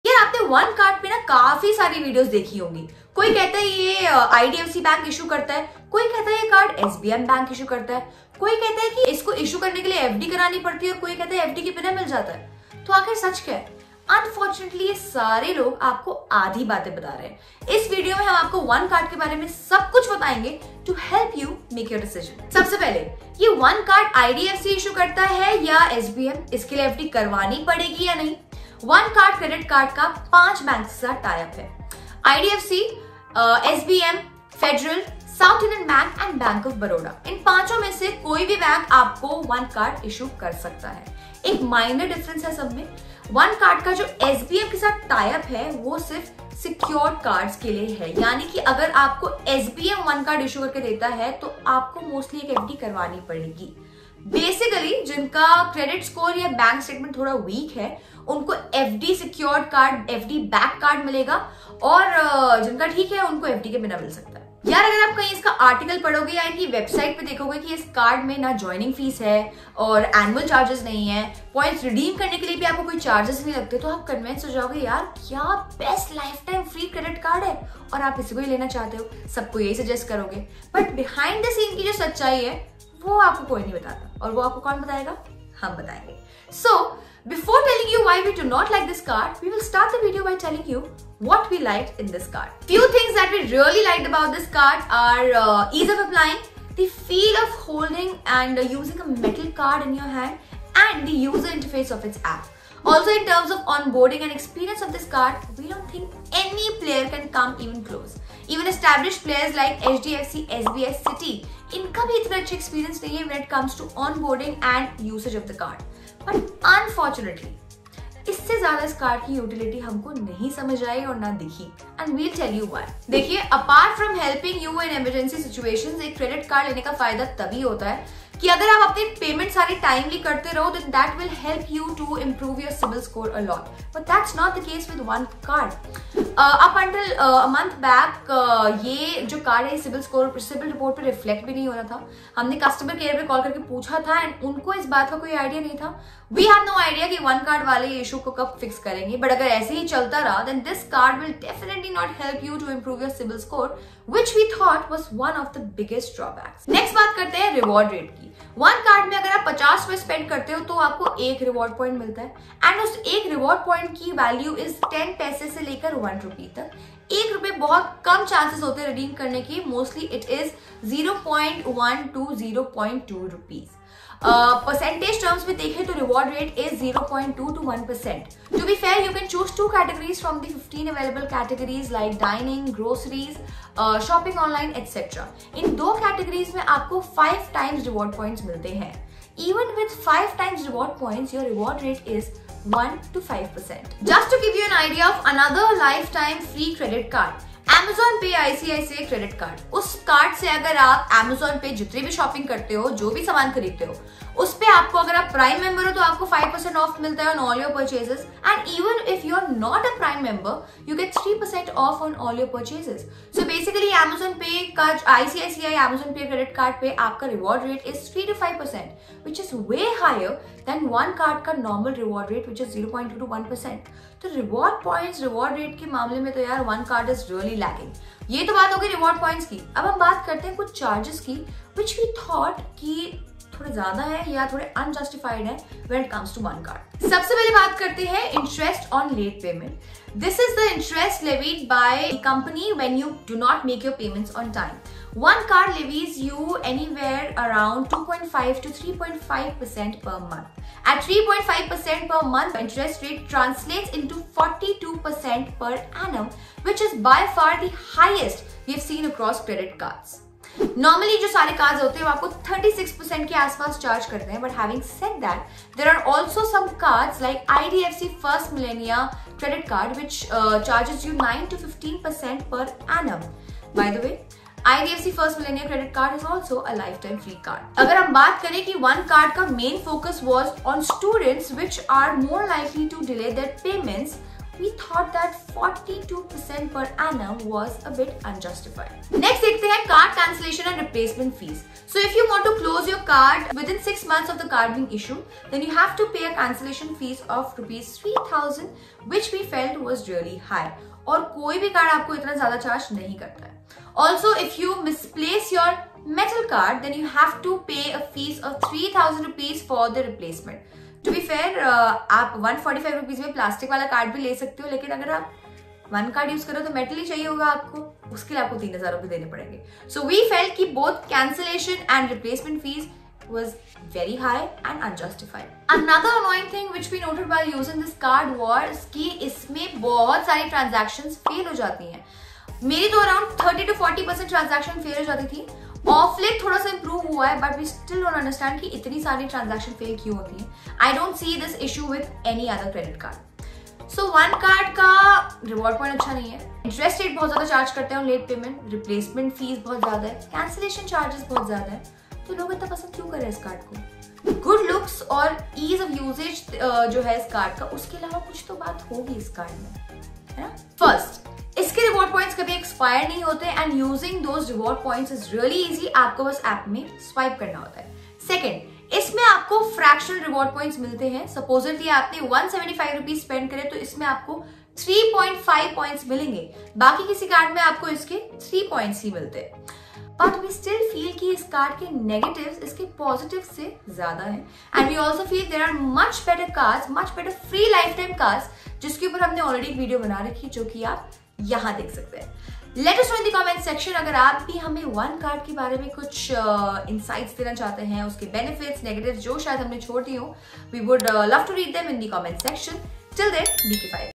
One Card Credit Card Honest Review || Reality of OneCard Metal Card Revealed
One Card Credit Card is one of India's First Fintechs to offer Metal Cards to its customers for ZERO Charges. Though this card has a lot going - Neat User Experience, Swanky Card, 5X Reward Points for top 2 categories, there's a lot missing also - even for a Lifetime Free Credit Card.

In the Video we highlight all the pros and cons of this card. Watch this video to decide if you should apply for this card or not

Follow our channels (bekifaayati) on